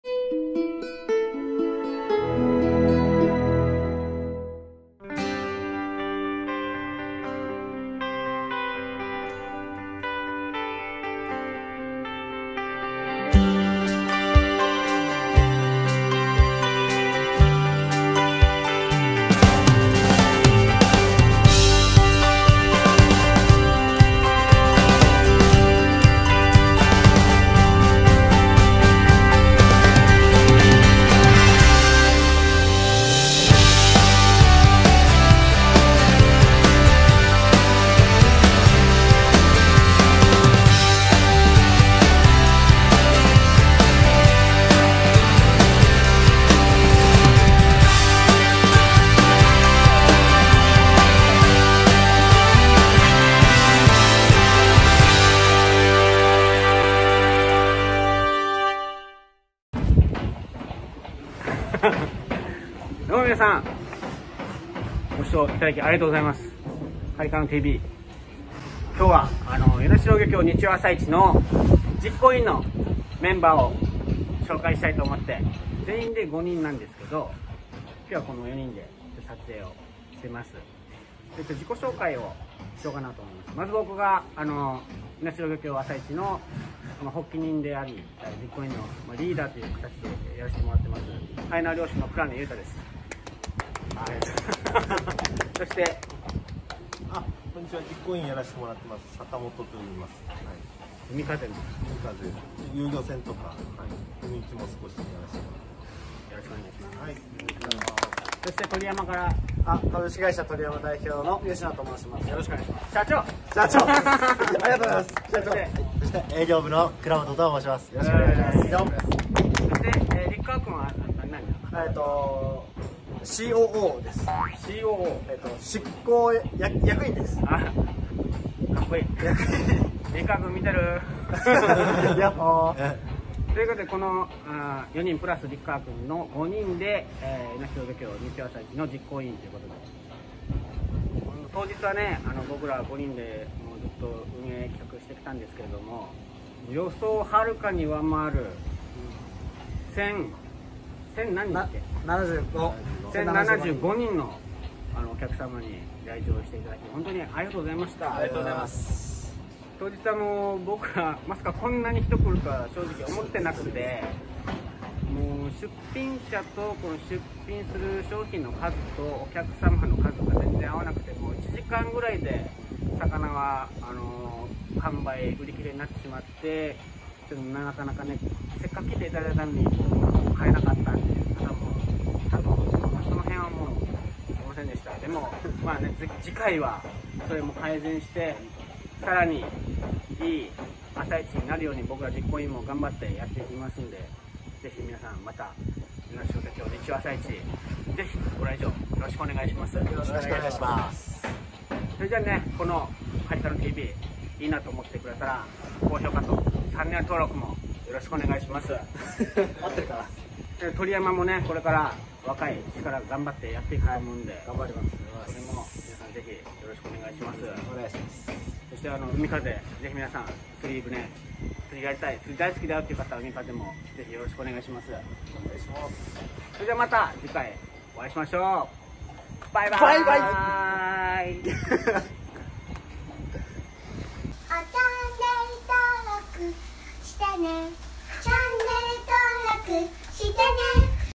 与那城漁協日曜朝市初開催！！
遂に初開催された与那城漁協日曜朝市！！当日はまさかの1000人越えの来場者！！

お客様の喜んでる姿を見る事が出来て、全てが報われた1日でした。

この日を迎えるために一年かけて準備してきました。最高の実行委員メンバーと一緒に成し遂げた達成感は一生の思い出です。

また次回の開催に向けて、さらに良くなるように実行委員一同頑張っていきますので、引き続きよろしくお願いします！

ブログ うちなークハルで応援して頂きました